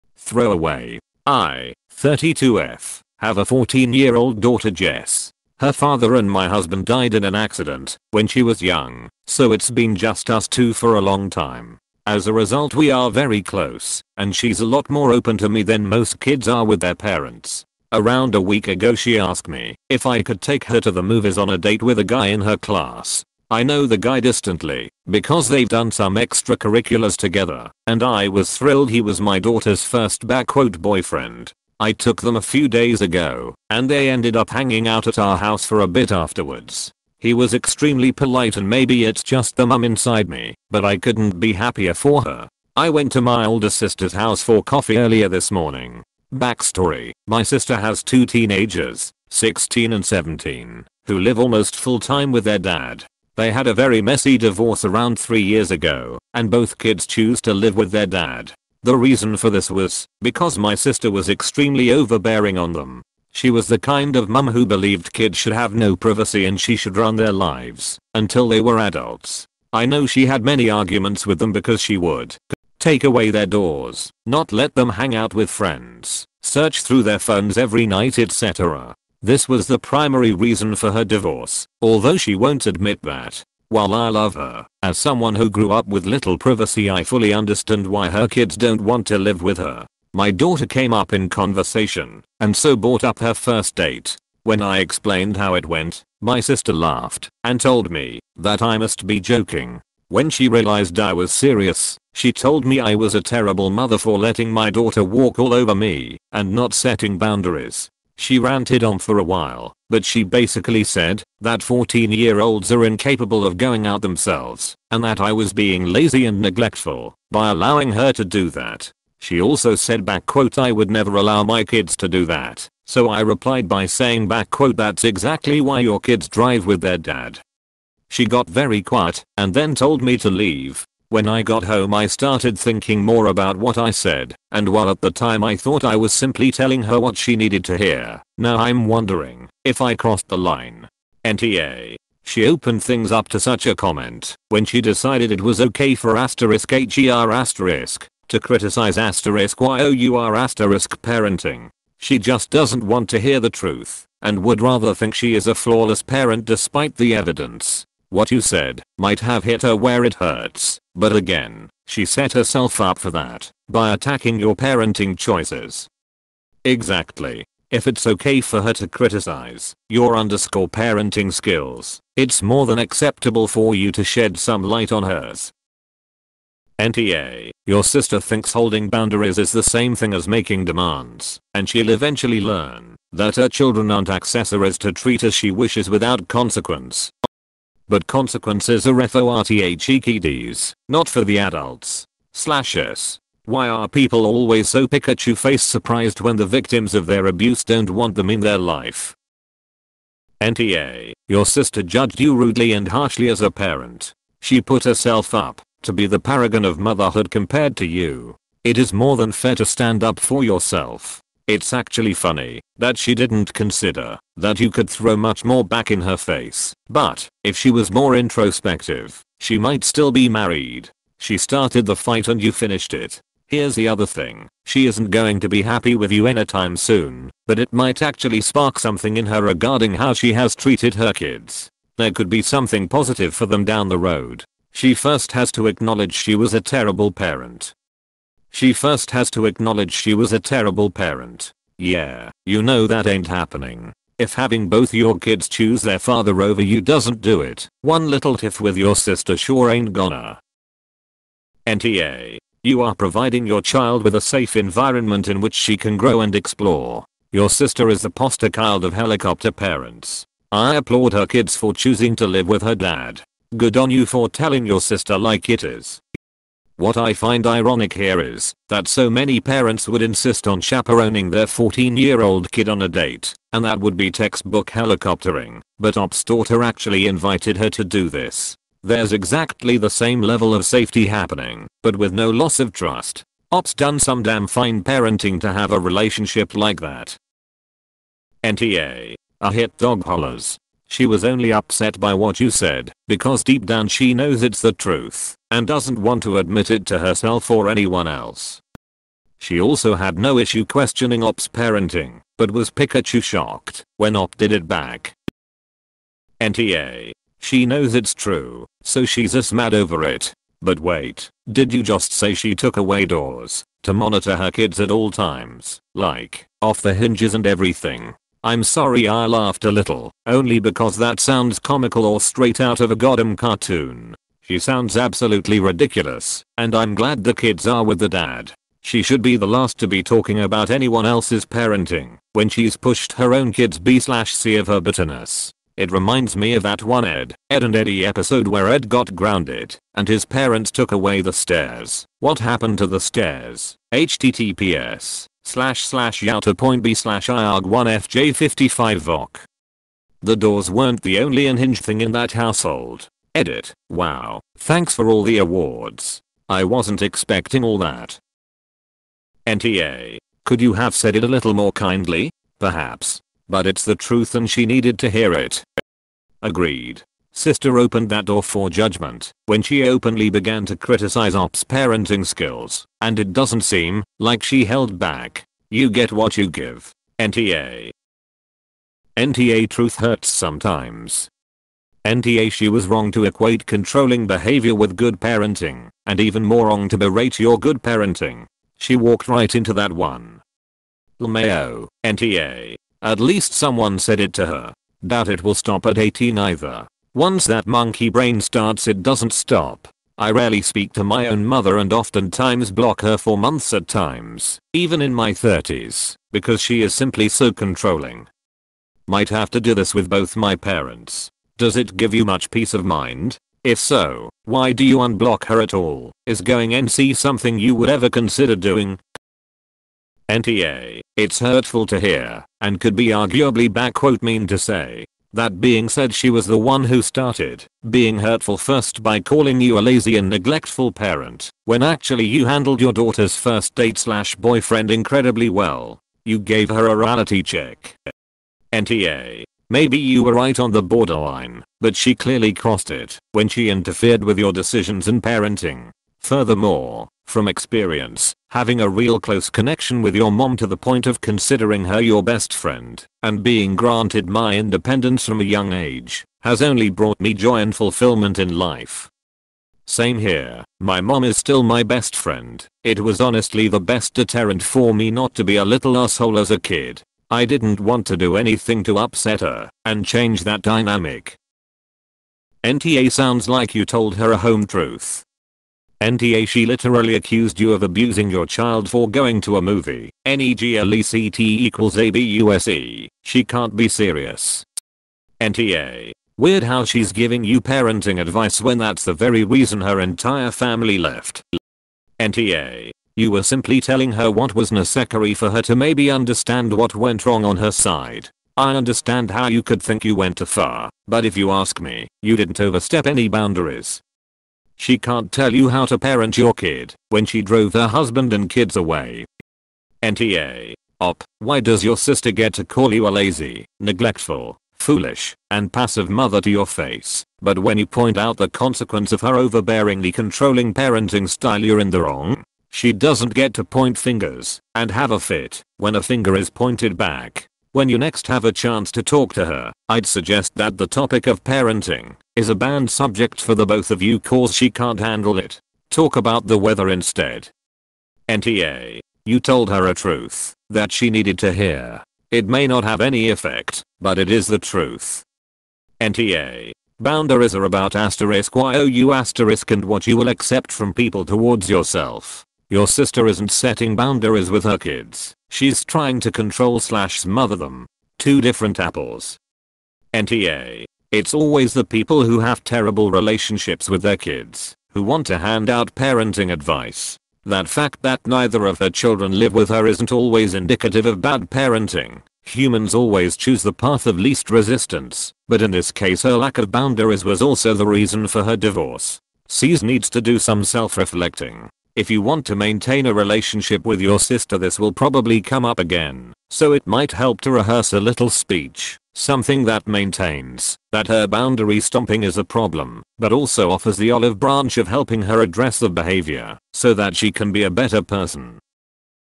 Am I the asshole for telling my sister there’s a reason her kids chose to live with their dad?
Posted in AmItheAsshole by u/throwaway0292720.